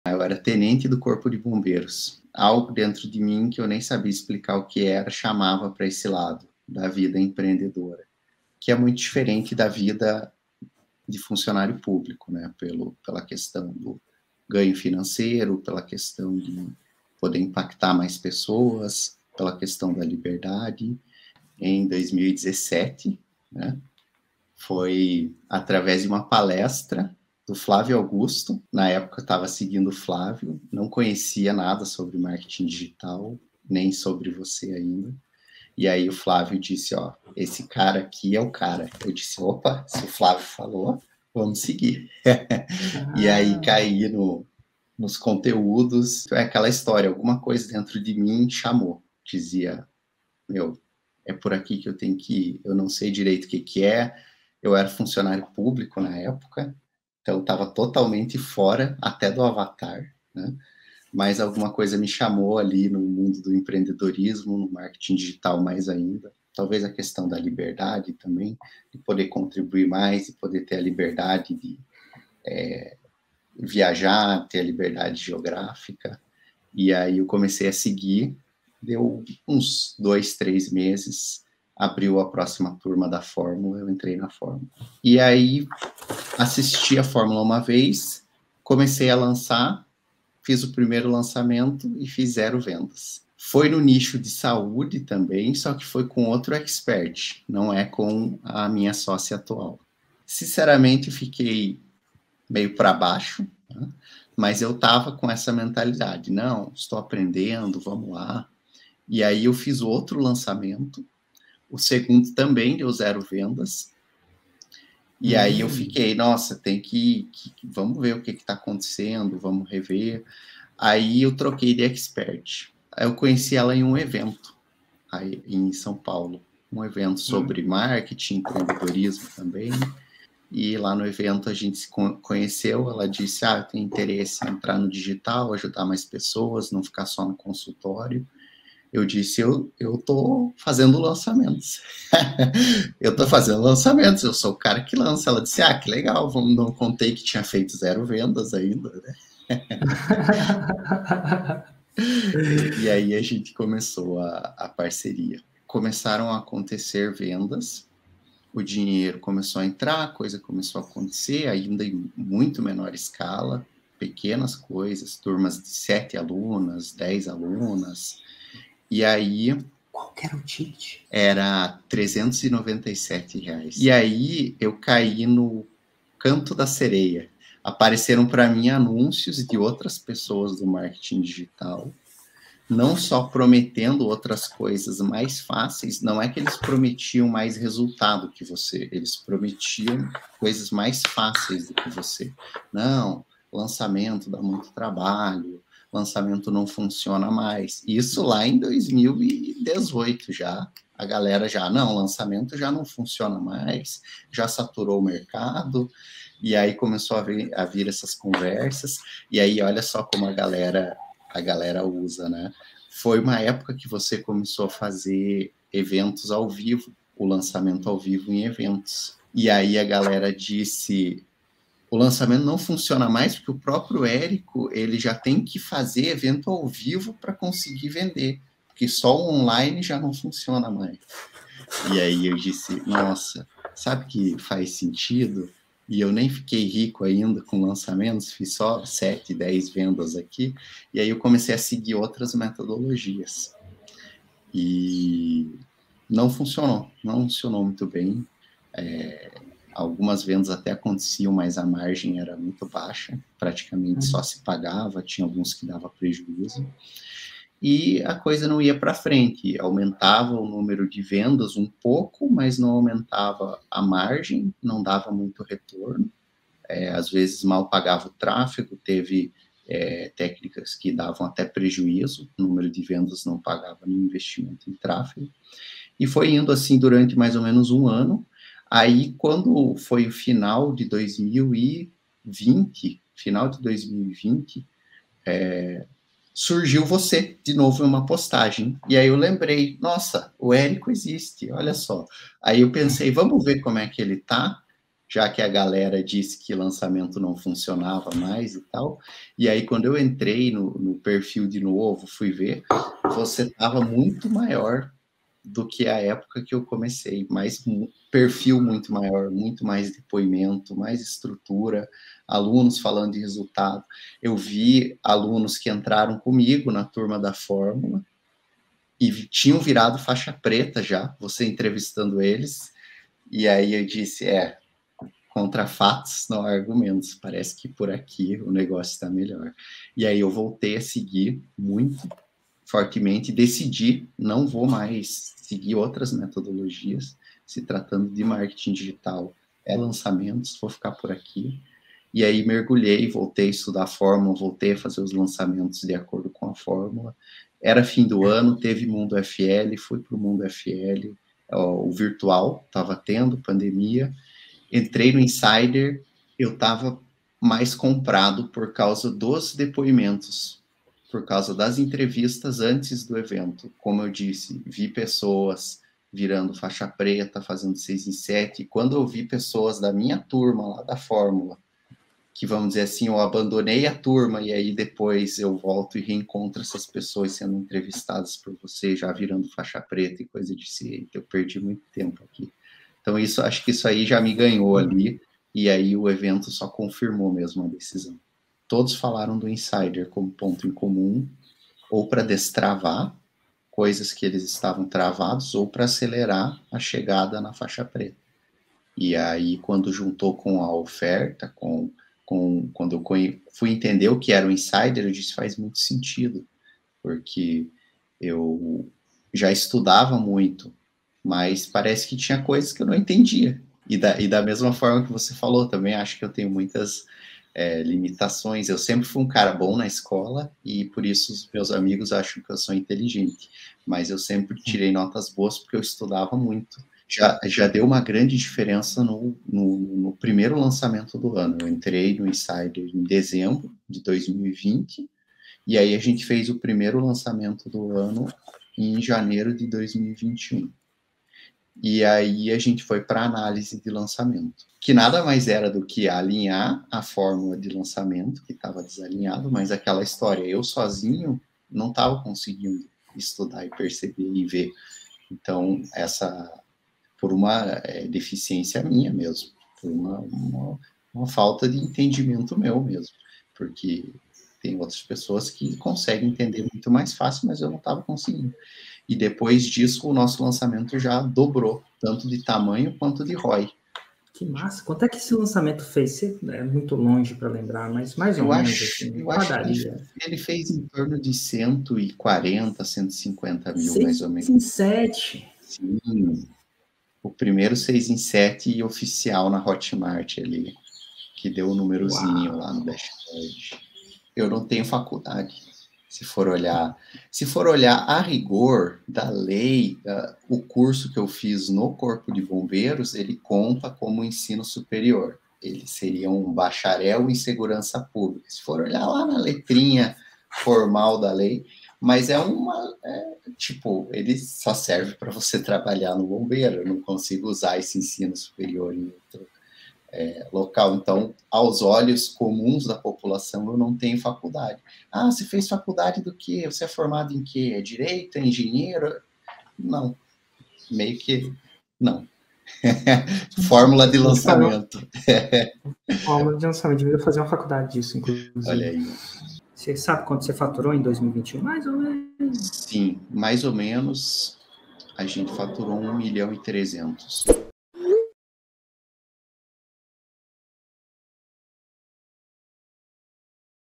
COMO ESSE EX-TENENTE DOS BOMBEIROS VIROU FAIXA-PRETA NO NICHO DE SAÚDE | ERICO ROCHA
Ele buscava liberdade financeira e geográfica, a metodologia da Fórmula de Lançamento proporcionou isso e mais um pouco.